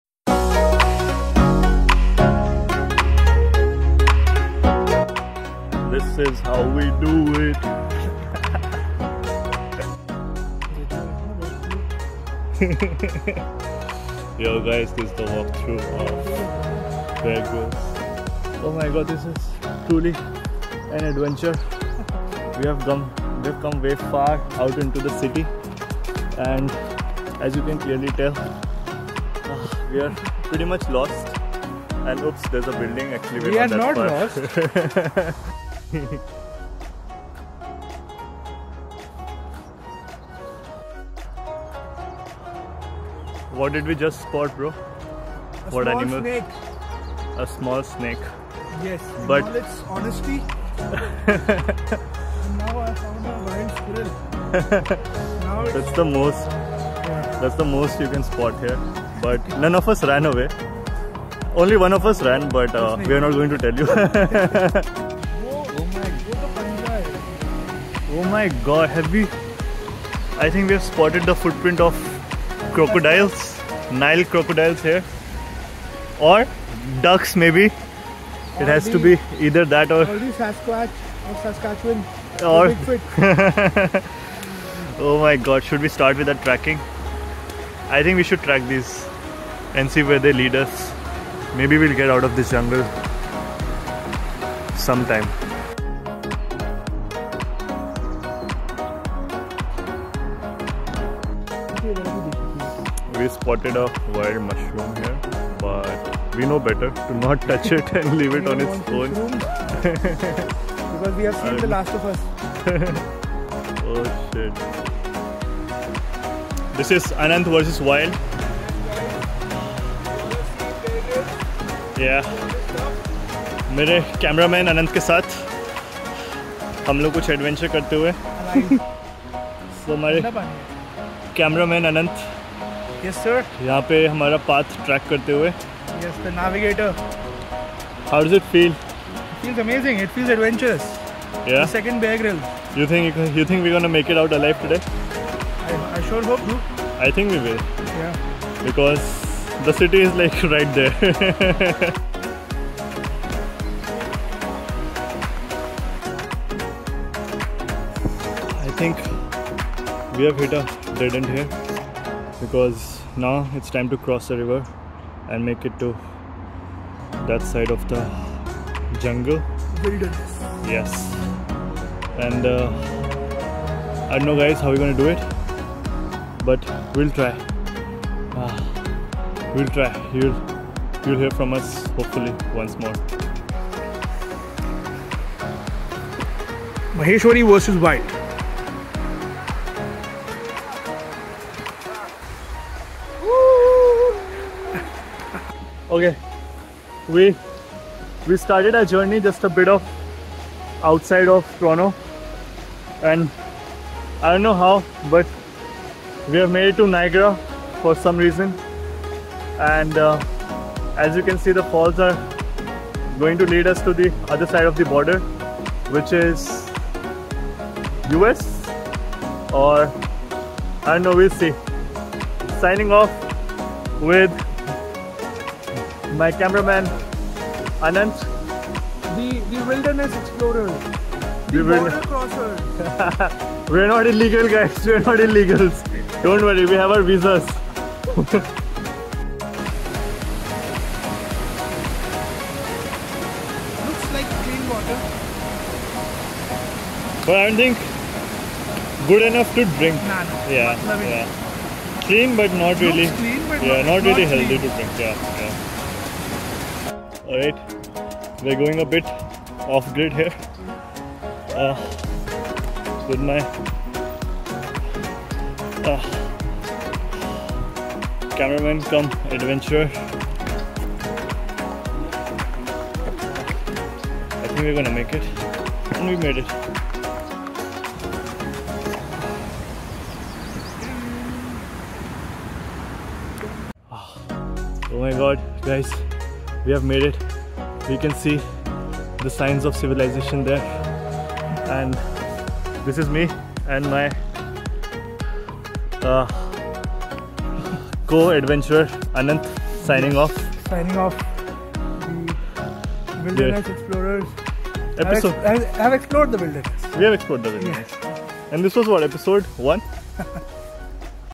We explored Crothers Woods and came across a snake (yikes)
Crothers Woods is an area of the Don River Valley in Toronto, Ontario, Canada. It is 52 hectares and consists of woodland, meadows, wetlands, and an assortment of past and present municipal uses.

I went with my friend to Crothers Woods for a hike and had lots of fun. Enjoy watching.